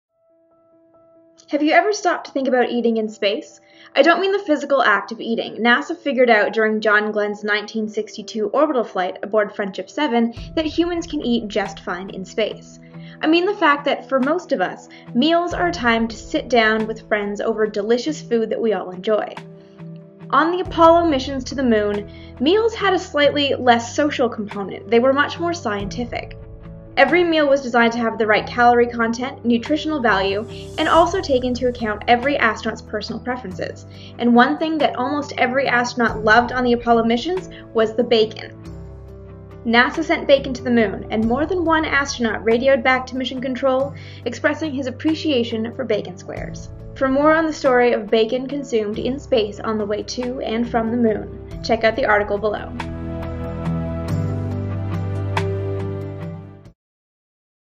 NASA Flew Bacon to the Moon
Among all the meals and snacks that accompanied the Apollo astronauts to the Moon was bacon. For the full story, check out the article at Vintage Space